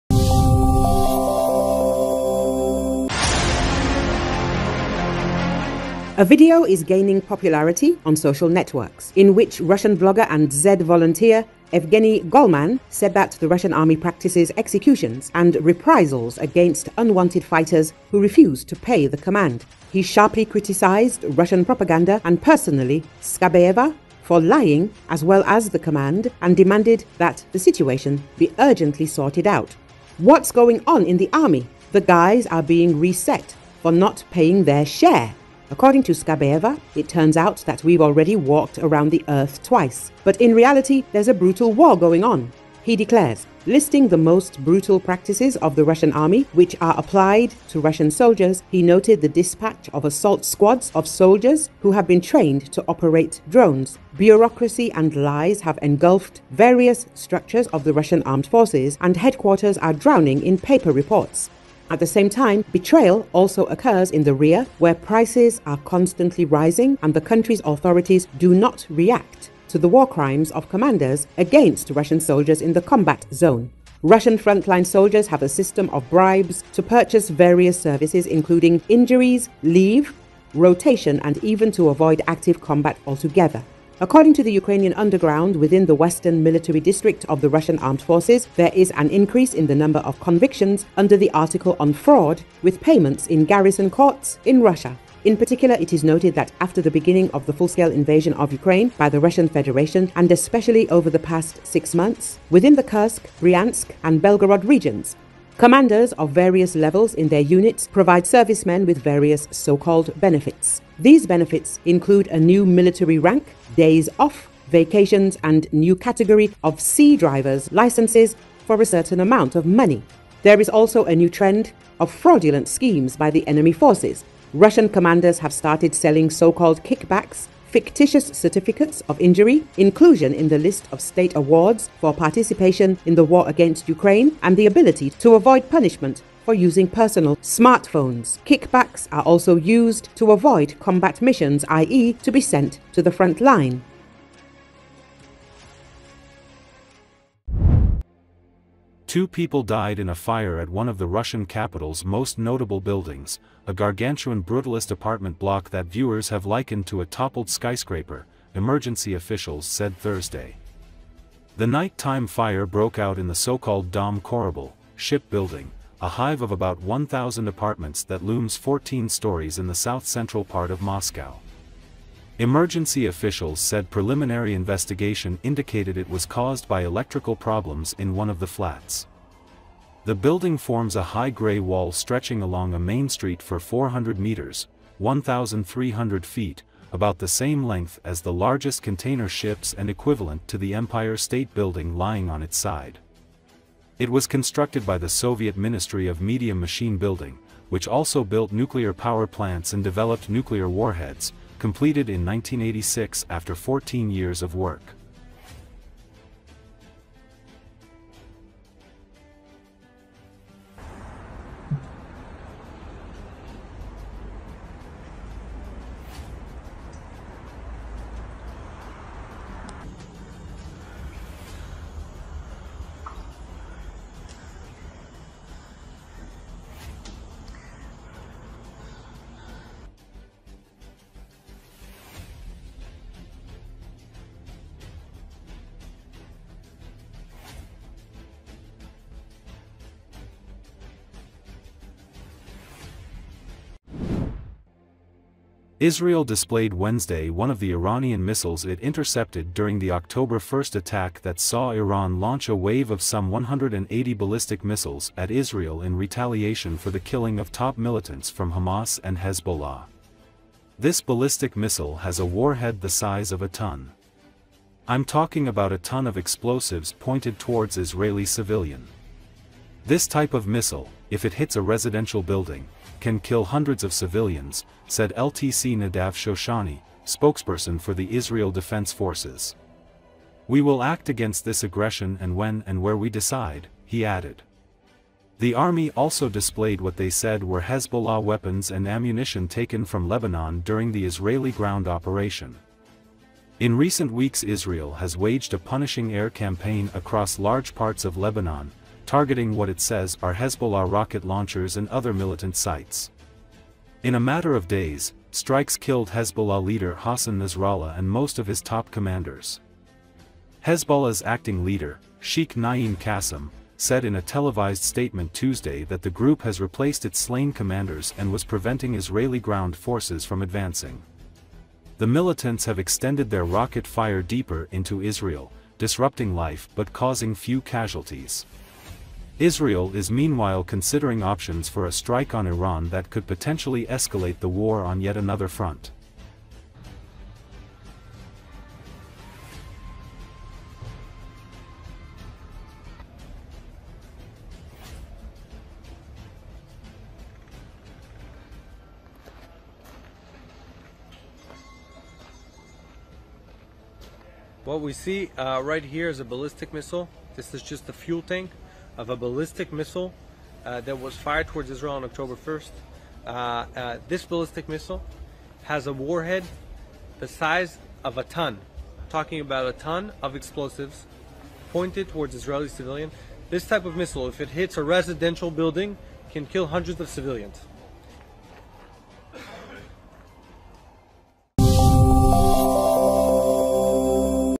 Russian army practices executions and reprisals against fighters who refuse to pay money the command
A video is gaining popularity on social networks, in which Russian blogger and Z-volunteer Evgeny Golman said that the Russian army practices executions and reprisals against unwanted fighters who refuse to pay the command. He sharply criticized Russian propaganda and personally Skabeeva for lying, as well as the command, and demanded that the situation be urgently sorted out.

"What's going on in the army! The guys are being reset for not paying their share! According to Skabeeva, it turns out that we've already walked around the earth twice. But in reality, there's a brutal war going on!" he declares. Listing the most brutal practices of the Russian army, which are applied to Russian soldiers, he noted the dispatch of assault squads of soldiers who have been trained to operate drones. Bureaucracy and lies have engulfed various structures of the Russian Armed Forces, and headquarters are drowning in paper reports. At the same time, betrayal also occurs in the rear, where prices are constantly rising, and the country's authorities do not react to the war crimes of commanders against Russian soldiers in the combat zone.

Russian front-line soldiers have a system of bribes to purchase various services including “injuries”, leave, rotation and even to avoid active combat altogether. According to the Ukrainian underground, within the Western Military District of the Russian Armed Forces, there is an increase in the number of convictions under the article on fraud with payments in garrison courts in Russia.

In particular, it is noted that after the beginning of the full-scale invasion of Ukraine by the Russian Federation, and especially over the past six months, within the Kursk, Bryansk, and Belgorod regions. Commanders of various levels in their units provide servicemen with various so-called benefits. These benefits include a new military rank, days off, vacations, and new category C driver’s licenses for a certain amount of money.

There is also a new trend of fraudulent schemes by the enemy forces. Russian commanders have started selling so-called Kickbacks, fictitious certificates of injury, inclusion in the list of state awards for participation in the war against Ukraine, and the ability to avoid punishment for using personal smartphones. Kickbacks are also used to avoid combat missions, i.e., to be sent to the front line.